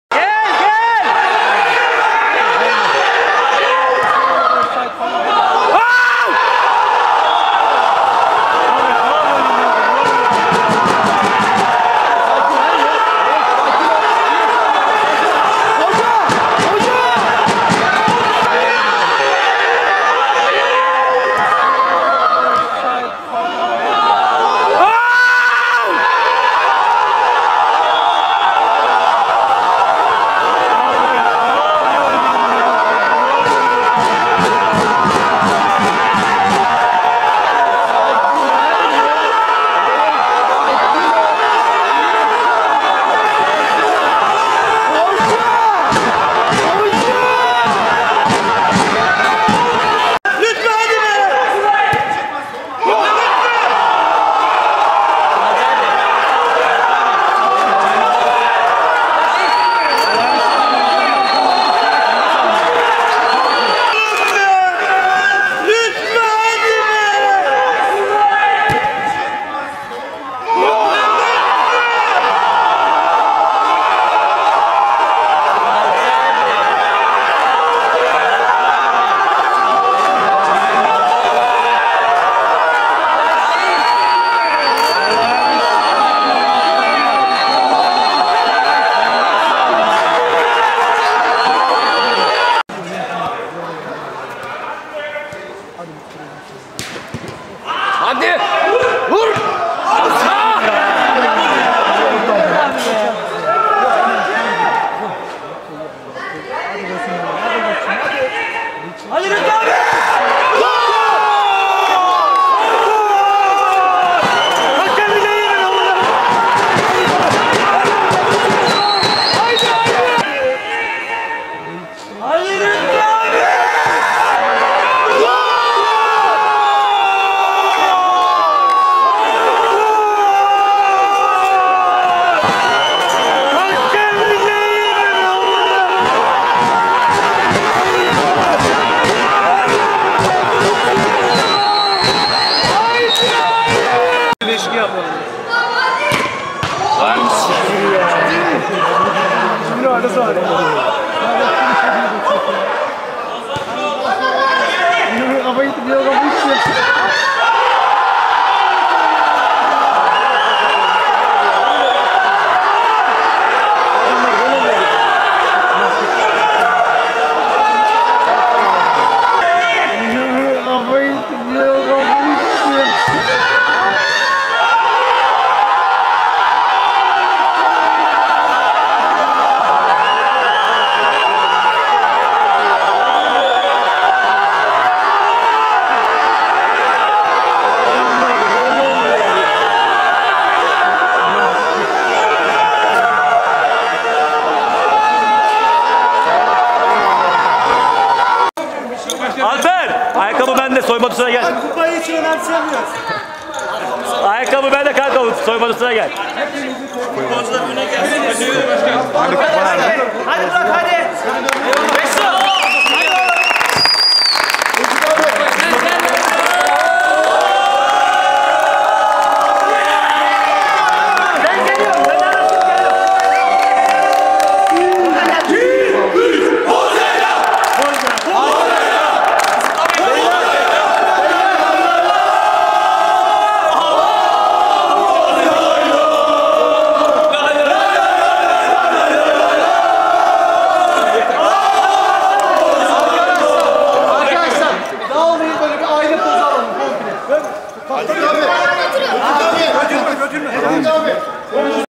Çorum Cup / Final Maçı Golleri Ve Kupa Kaldırma / Bozyayla - Turgutlu
ŞAMPİYON BOZYAYLA
Maç Sonucu : Bozyayla 3-1 Turgutlu